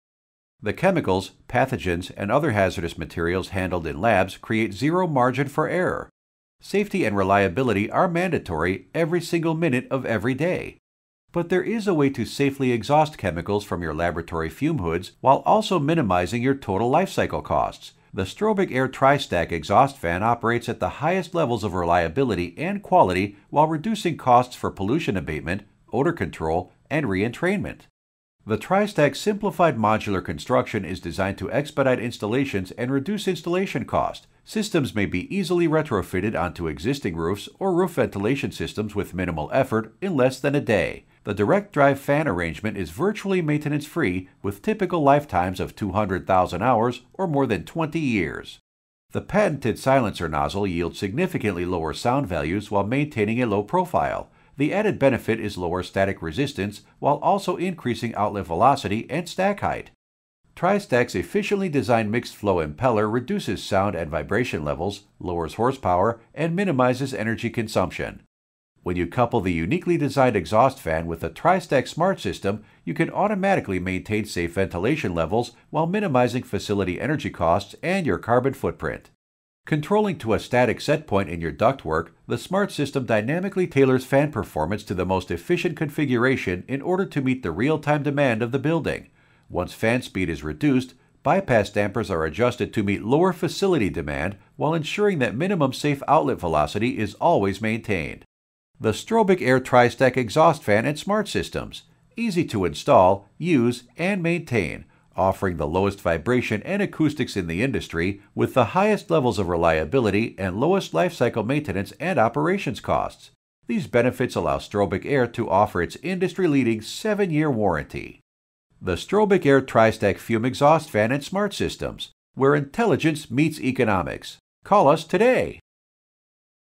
Strobic Air Tri-Stack® laboratory and industrial process fume exhaust systems
Strobic Air Tri-Stack® laboratory and industrial process fume exhaust systems are practical, cost-effective and energy efficient solutions for pollution abatement, re-entrainment and odor control problems. Strobic Air is a CECO Environmental brand.